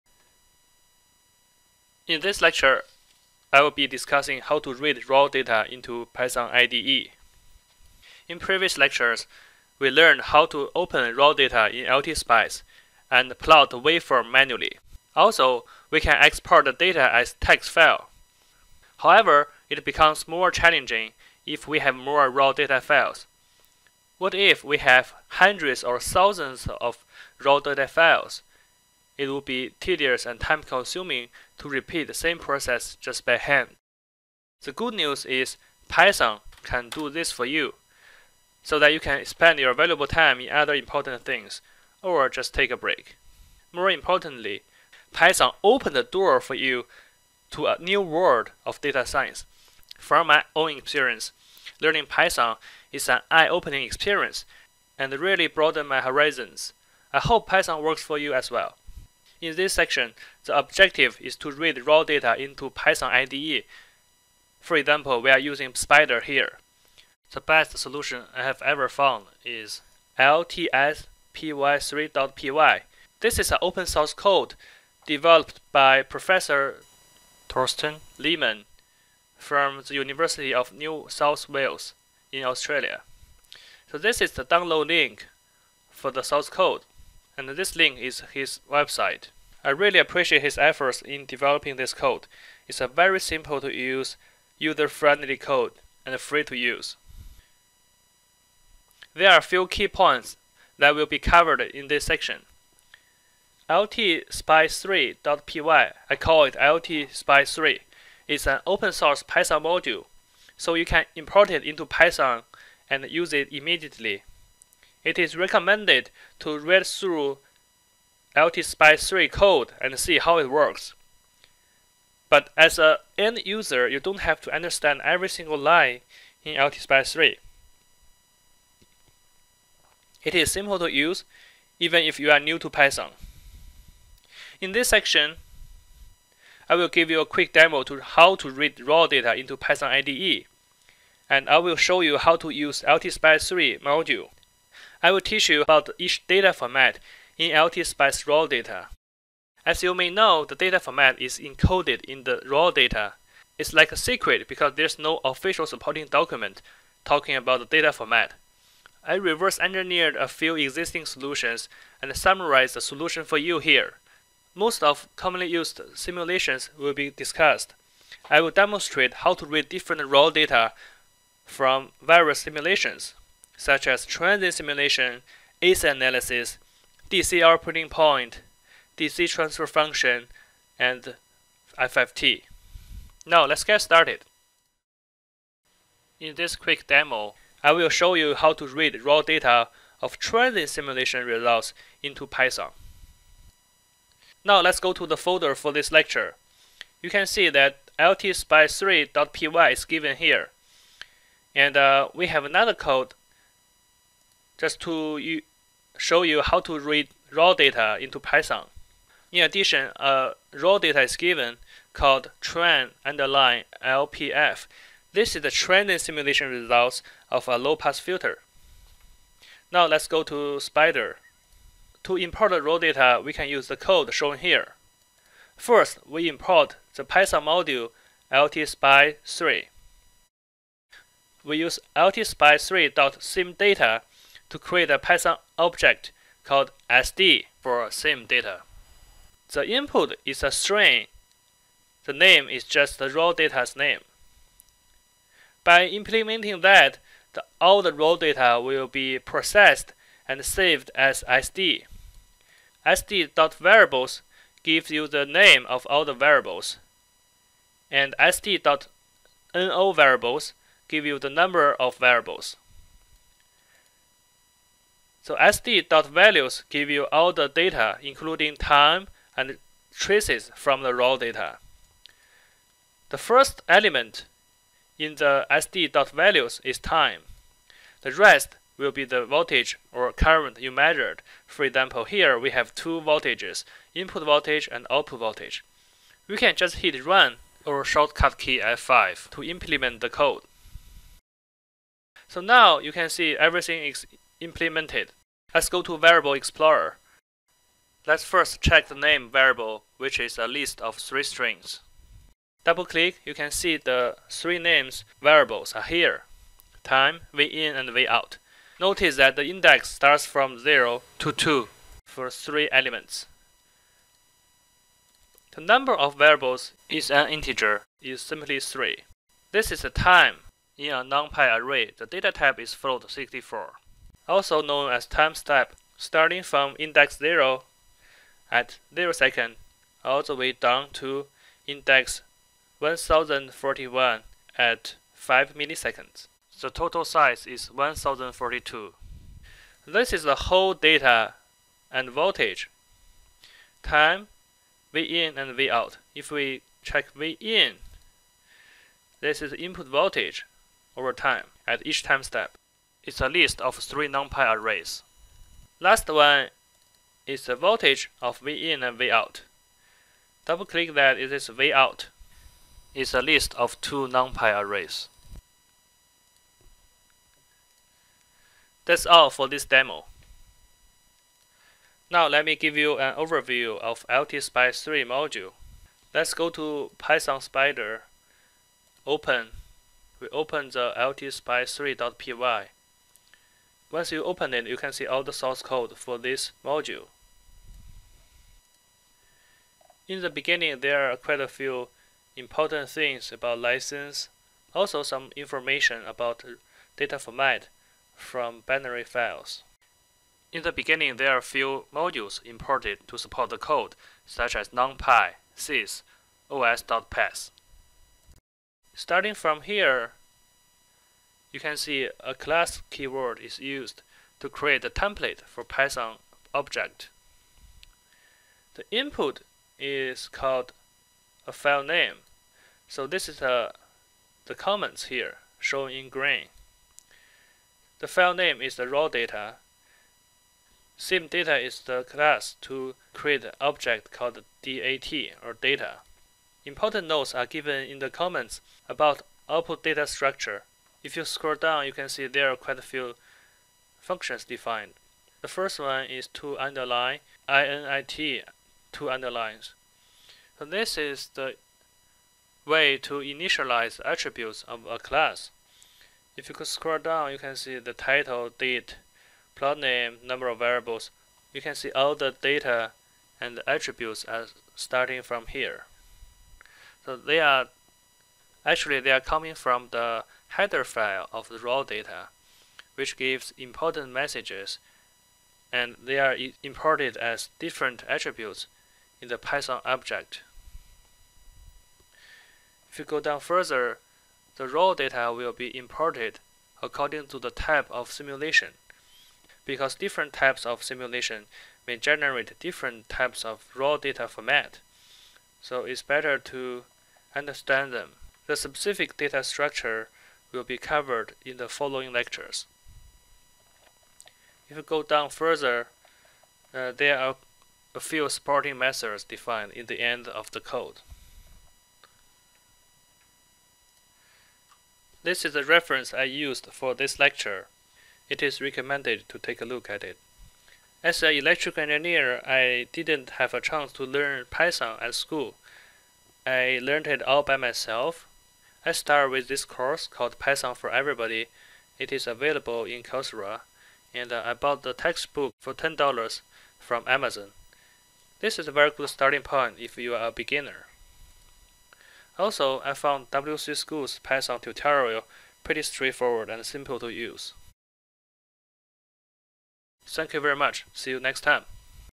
Introduction: read raw data from LTspice into Python IDE
Udemy course about "Automate LTspice Circuit Simulation Using Python Scripting"